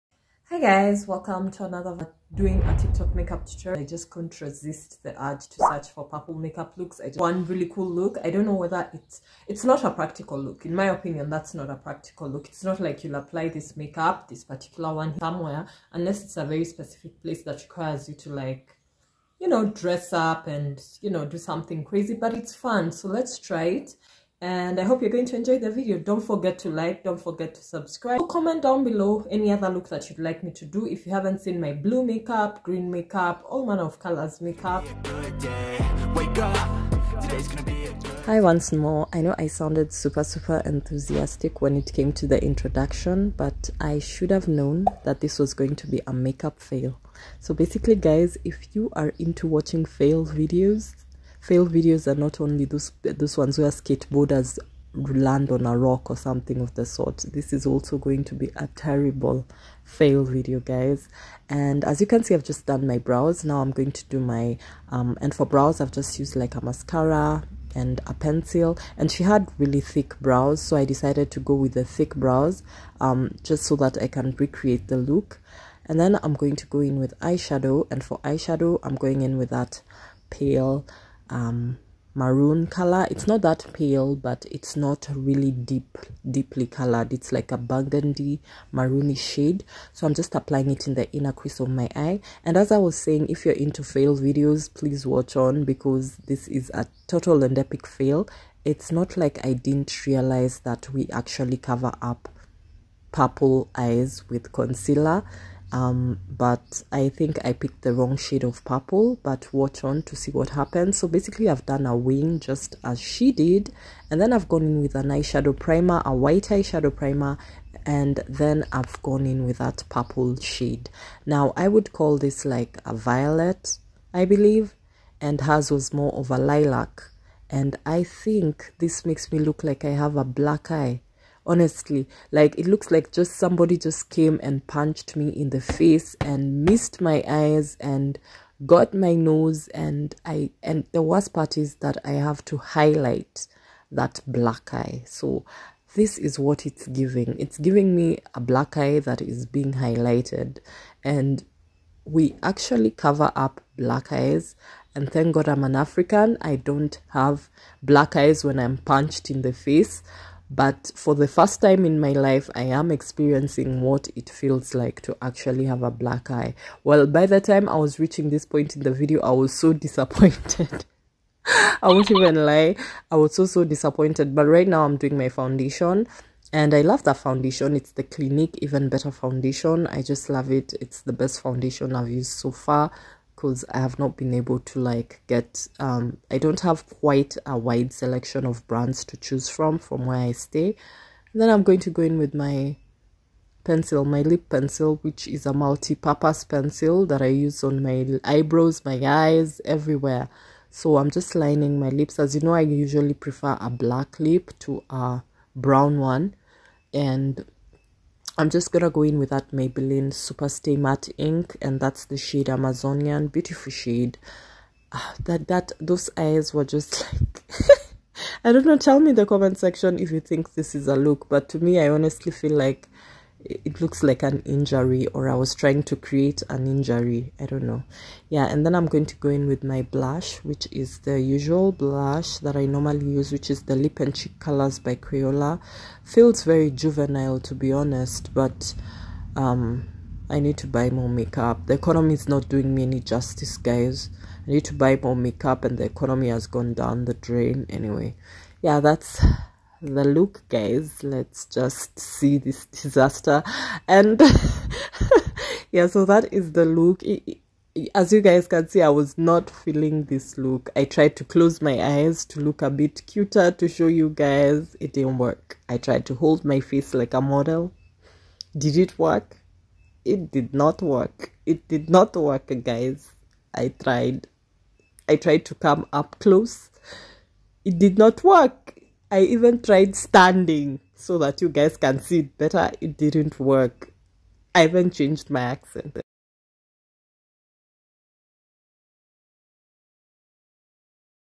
I TRIED A VIRAL TIKTOK 💄MAKEUP LOOK//IT MADE ME UGLIER☹️😬
Hi all,
here is another MAKEUP video.We are doing a Makeup look that is absolutely eye catching,pun intended again.But will this TIKTOK MAKEUP be eye catching on me??? lets find out.
products used to recreate this look

You May Also Enjoy The following Videos

You may also be like the following videos

~Instagram~@naningakoei
~twitter~@naningakoei
~tiktok~@naningakoei

8
Endebess-30201
Kenya